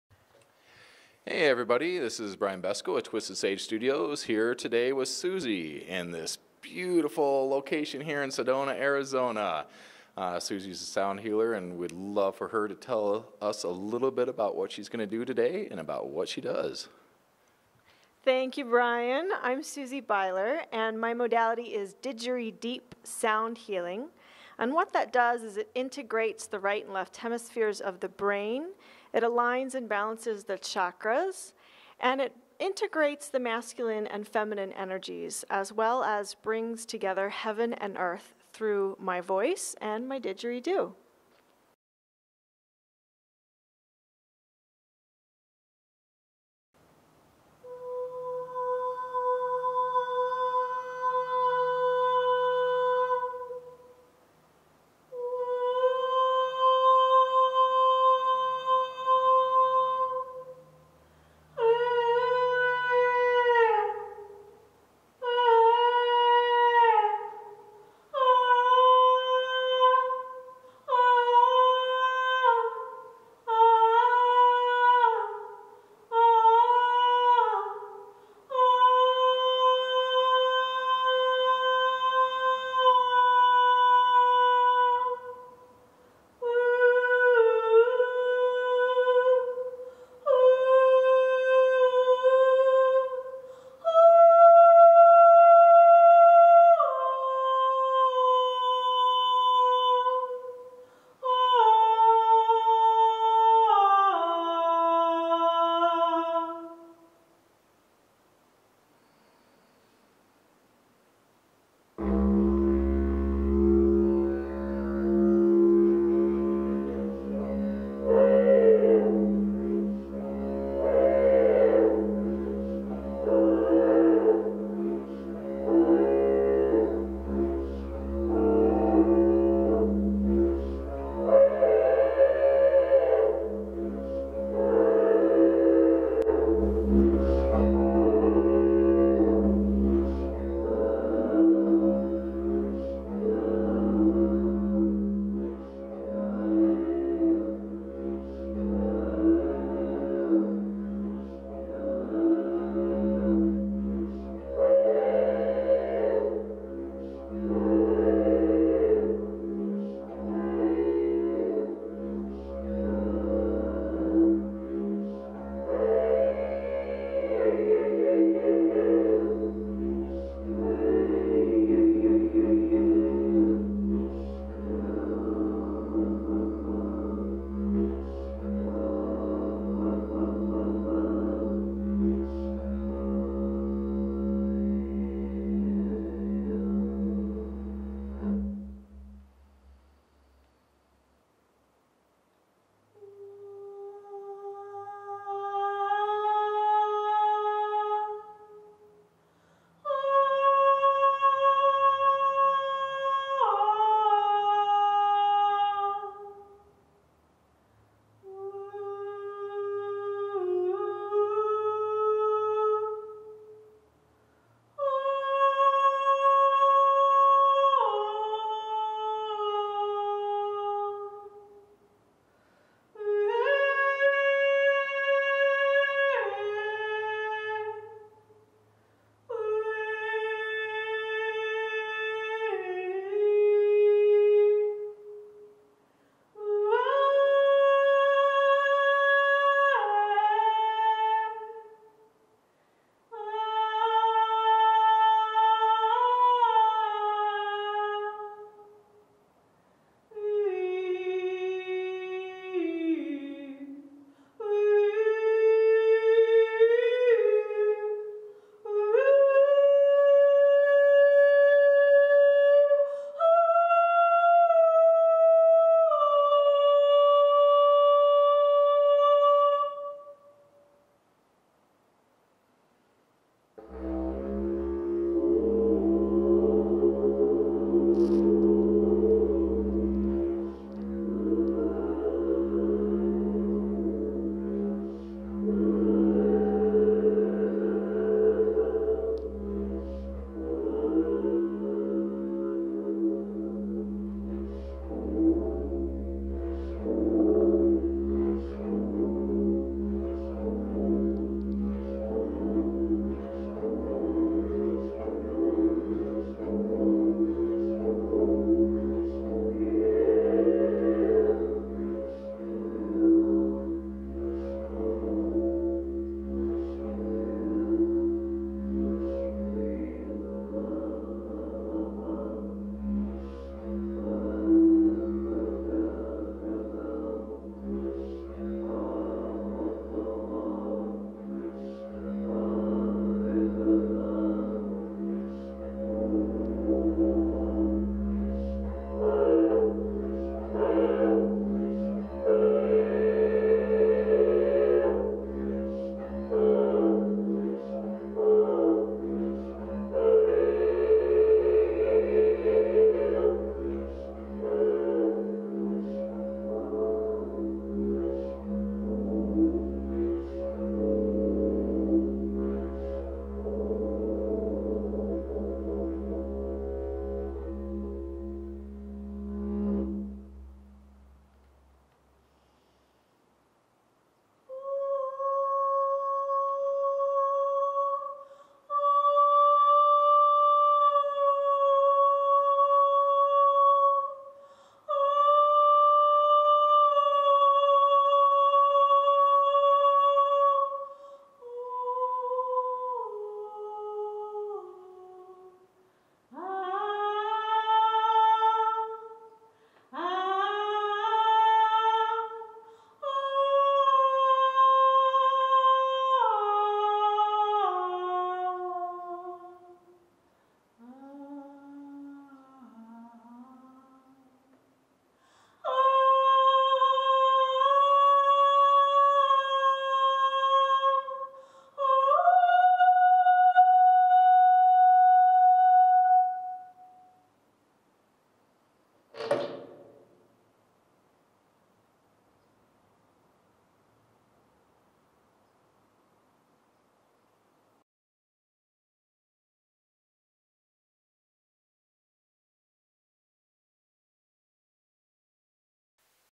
DidgeriDeep Sound Healing Session
Twistedsage Studios presents a look into Sound Healing with Susie Beiler through the use of her Didgeridoo and channeled Toning, which she has trademarked as DidgeriDeep Sound Healing.